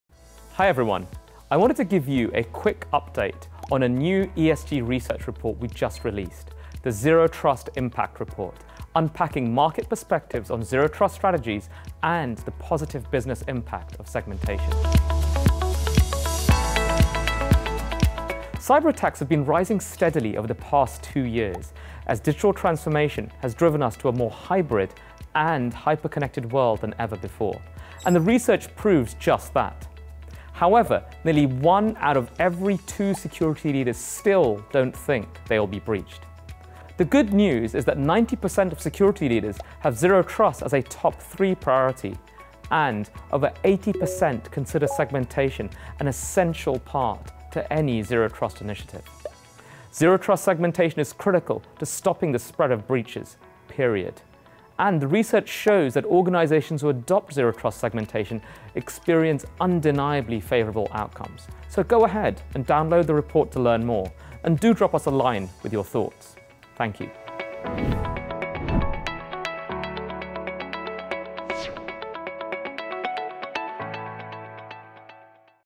Zero Trust Impact Report: ESG Research Highlights
Watch this quick overview of key research findings from the Zero Trust Impact Report by Enterprise Strategy Group (ESG).

Based on a survey of 1,000 IT and security professionals worldwide, the Zero Trust Impact Report explores how organizations prioritizing Zero Trust Segmentation achieve better business and security outcomes, including stronger cyber resilience, improved operational efficiency, and accelerated cloud adoption.